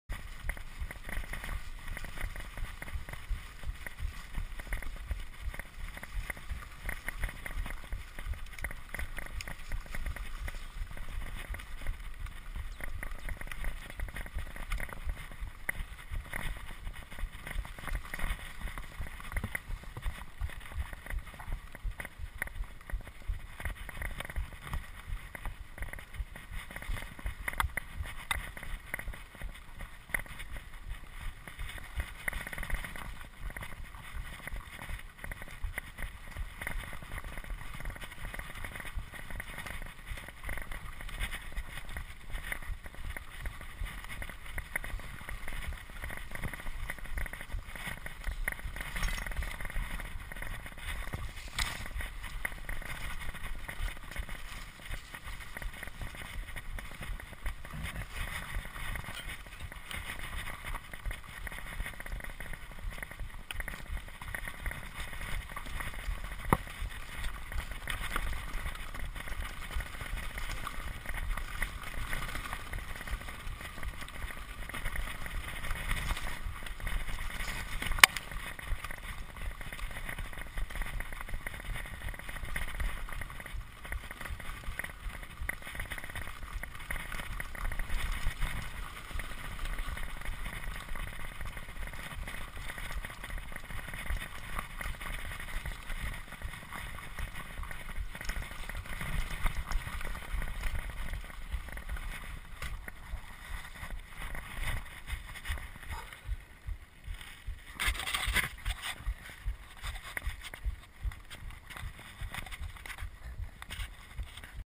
Hoch über dem Muttental
Auf der Klippe Richtung Ruhr. Am Ende leider eine Sackgasse.